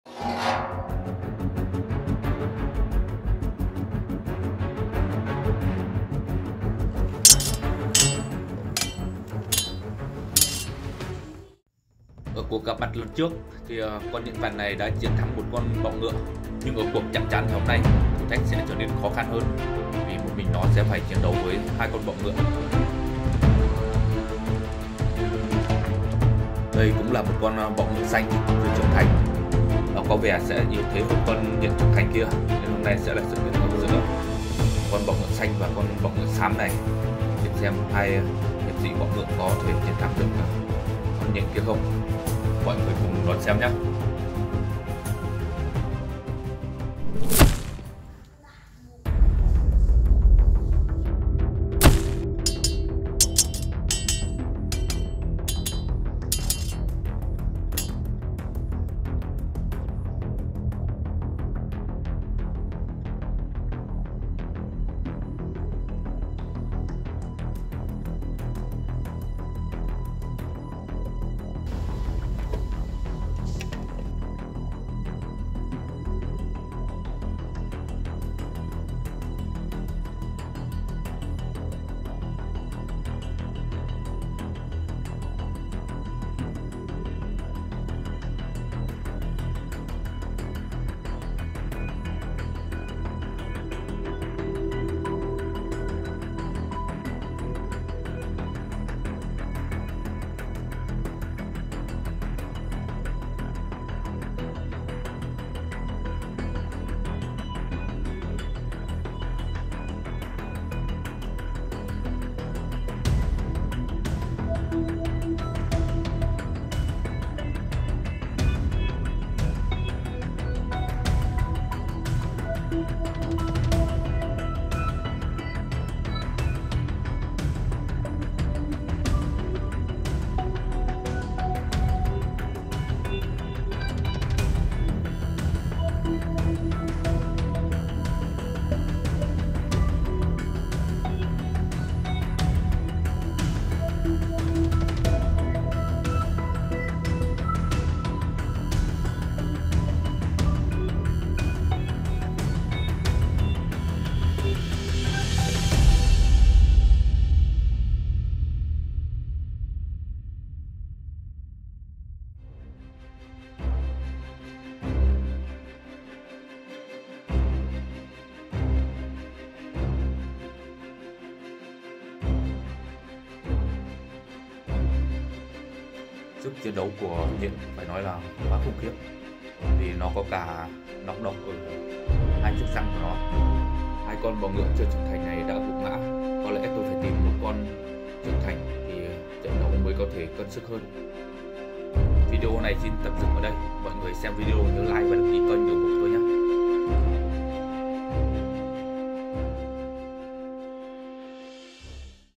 Unequal battle: 2 praying mantises - 1 zebra spider | Tropical Forest Bees
Venomous Spider